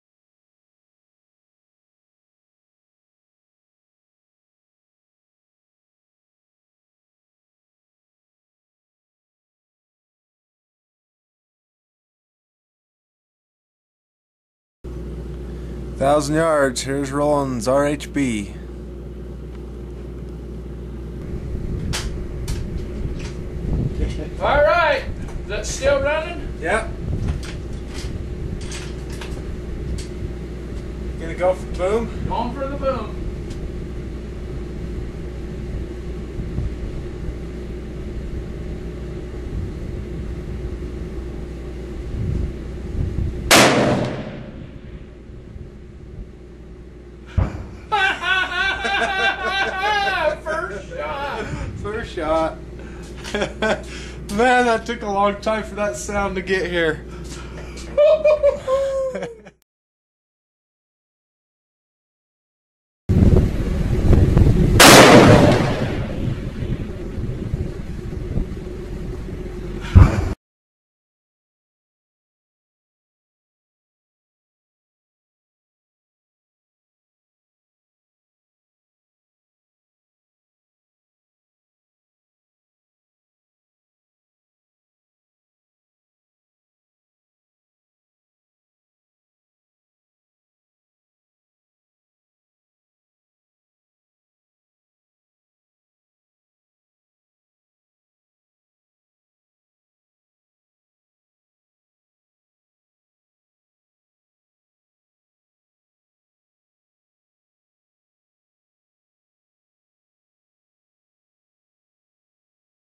1000 yard long range tannerite shot
1000 yard long range shot hitting a Tannerite Capsule the size of a salt shaker.  Creates quite the bang.  This shot was made with a custom built 7mm RHB rifle designed by me.

Comments are welcome.